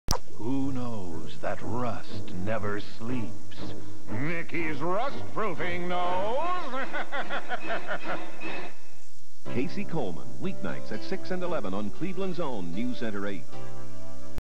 Partial Mickey's Rustproofing commercial & Casey Coleman Spot WJW 1991
Excuse the poor tape quality. All copyrights acknowledged. No infringement is intended.